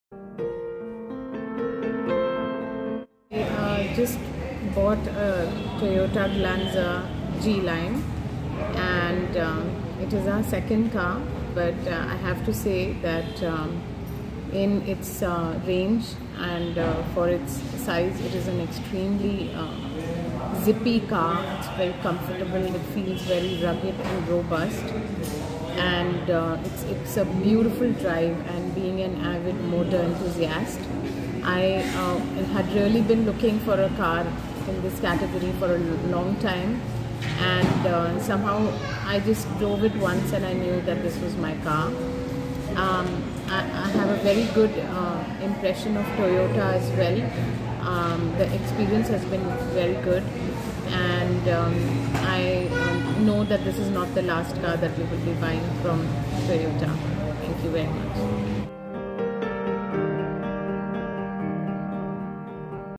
Heart Touching Guest Experience at Nandi Toyota - Mrs. Bhavana Gupta
Namaste!!

Welcome to Nandi Toyota Family,

Mrs. Bhavana Gupta, one of our most proud family members, expressed her appreciation for her overall experience with Nandi Toyota, including the Sales, Service, and Exchange process parameters, from the time she purchased her first Toyota until today.

Thank you for being our valued customer. We are so grateful for the pleasure of serving you and hope we always meet your expectations.